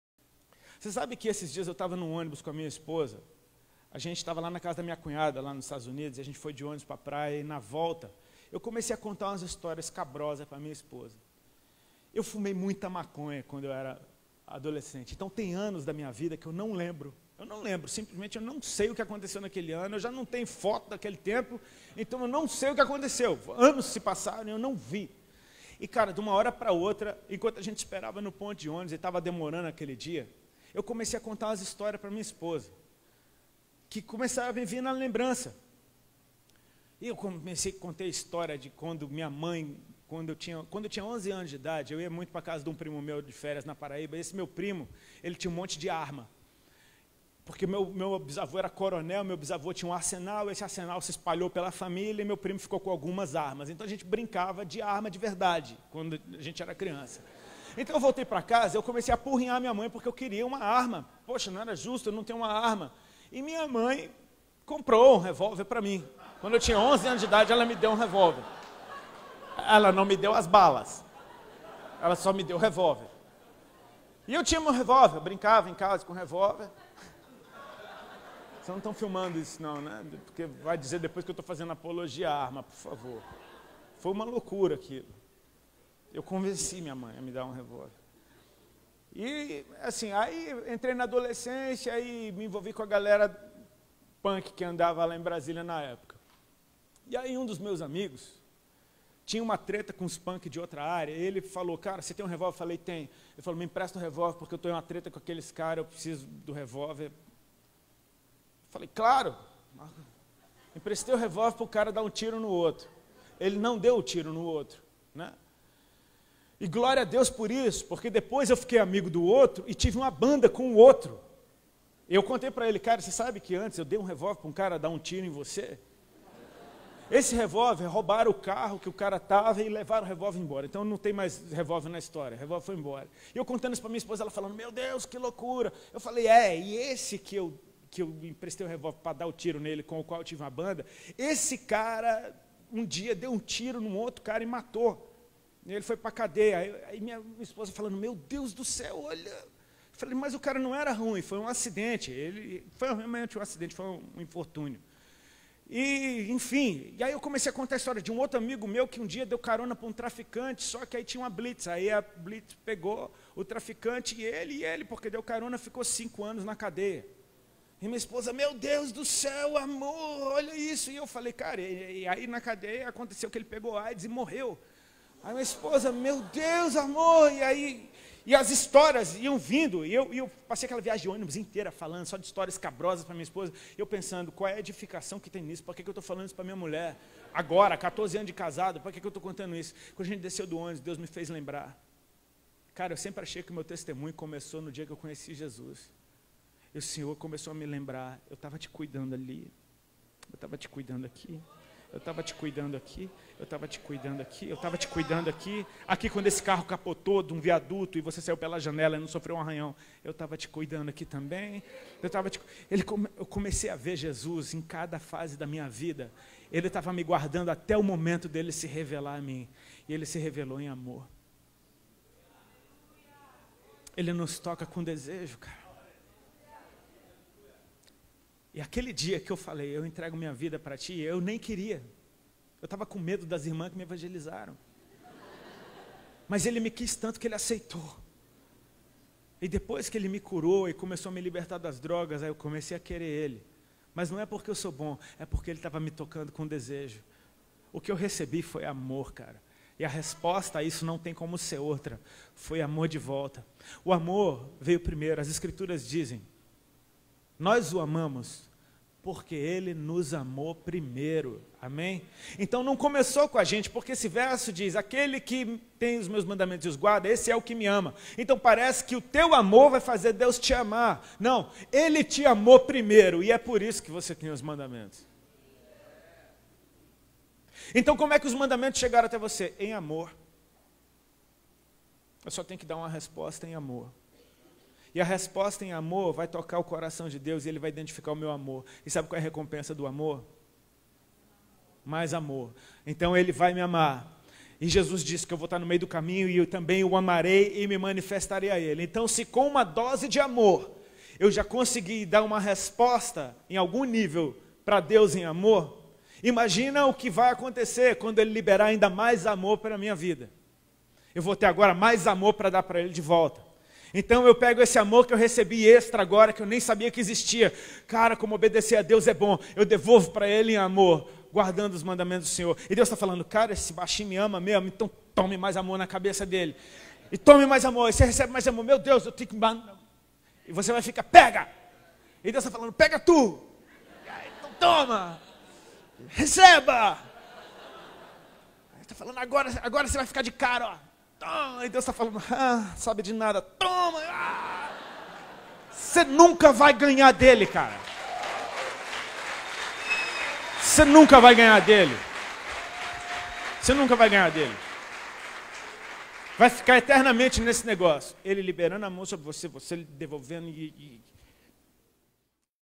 O amor de Deus - Rodolfo Abrantes
Mensagem minstrada por Rodolfo Abrantes na Conferência JesusCopy2015 que aconteceu em Bragança Paulista/SP.